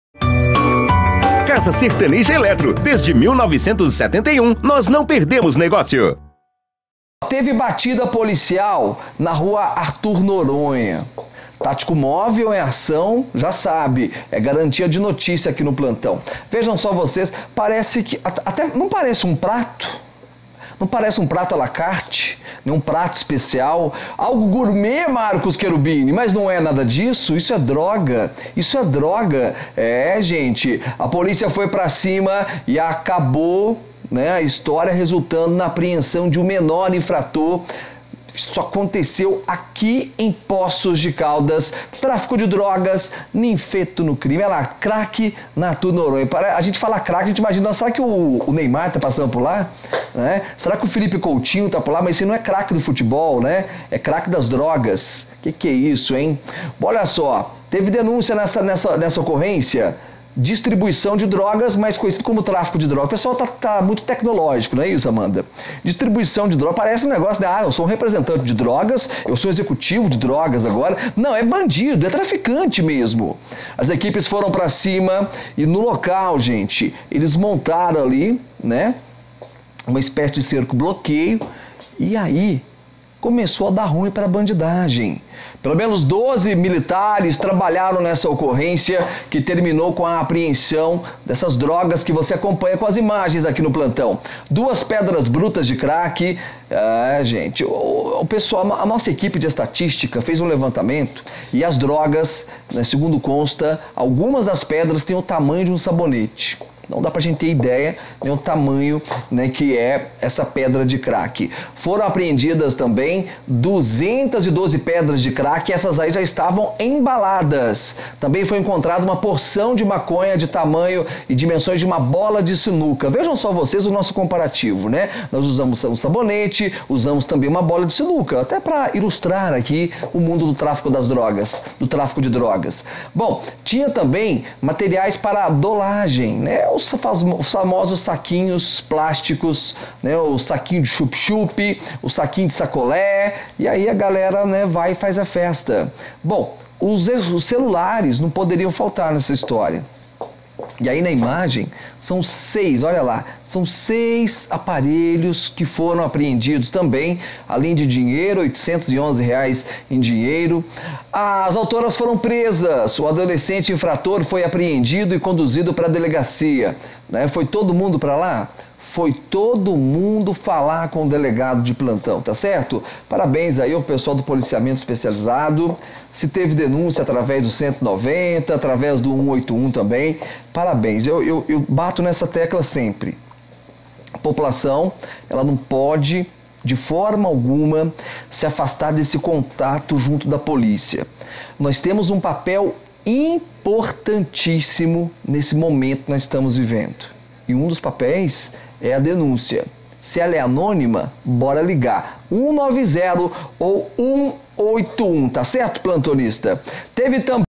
TRÁFICO ARTHUR NORONHA
OPERAÇÃO PRENDE DUAS MULHERES E APREENDE UM MENOR SUSPEITOS DE TRÁFICO DE DROGAS

Equipe do Tático prende duas mulheres e apreende um menor suspeitos de tráfico de drogas durante Operação Batida Policial na rua Arthur Noronha em Poços de Caldas.